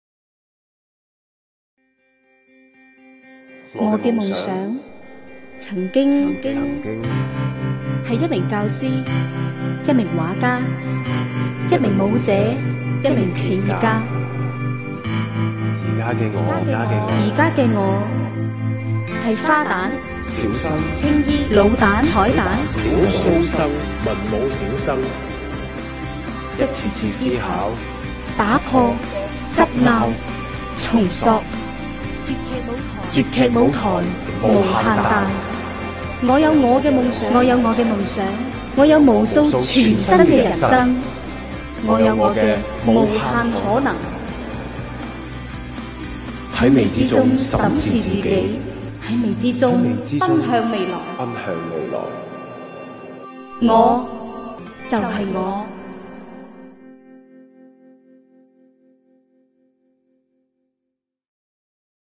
粵劇 廣東粤劇院《2021 中青年演藝大賽》於(2021.07.28)至(2021.07.31) 舉行 ，廣東一團及二團的中青年演員包括許文杰(許歡歡)及楊静將參予首場演出 canton opera
粤劇資訊
廣東粤劇院 2021中青年演藝大賽，將於2021年7月28日至7月31日舉行，参予者包括廣東一團及二團的中青年演員，其中許文杰(許歡歡)及楊静在首場分別飾演許仙及百花公主等。